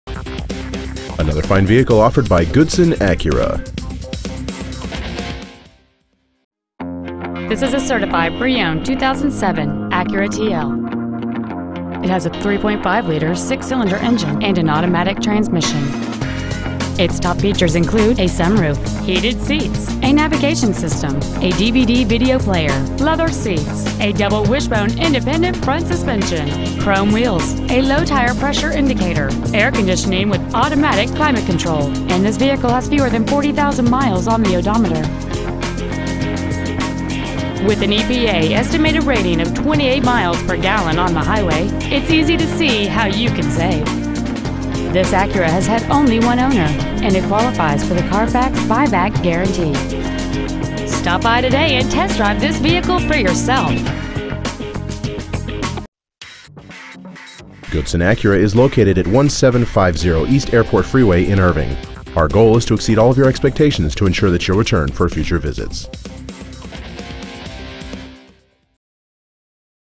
Used 2007 Acura TL Nav Irving TX
We are proud to present this 2007 Acura TL Nav.

 Year: 2007
 Make: Acura
 Model: TL Nav
 Engine: 3.5L SOHC PGM-FI 24-valve VTEC V6 engine
 Trans.: Automatic
 Exterior: Blue
 Miles: 39,959
 Interior: Gray

 Pre-Owned 2007 Acura TL Nav Irving TX